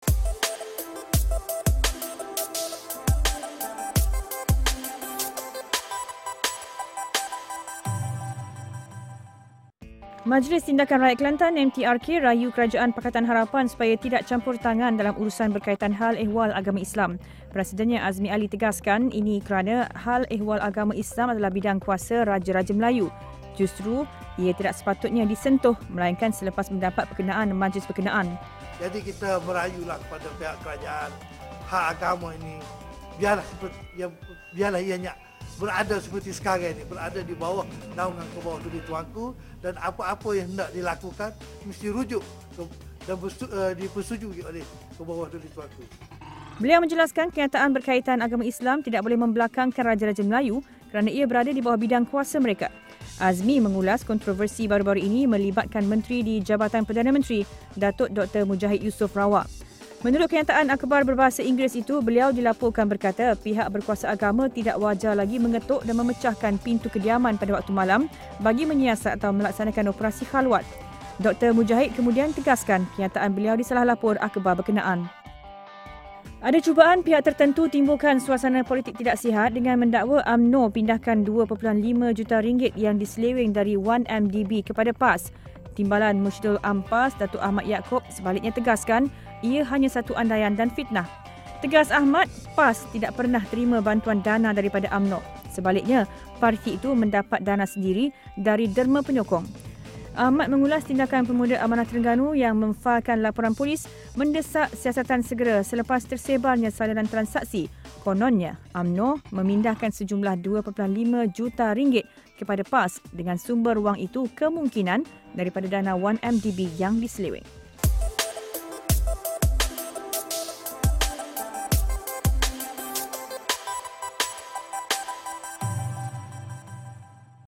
AWANI - Kelantan: Ada pihak cuba timbulkan suasana politik tidak sihat
AWANI - Kelantan: Ada pihak cuba timbulkan suasana politik tidak sihat.Laporan berita dan kupasan isu setiap negeri-negeri di Malaysia.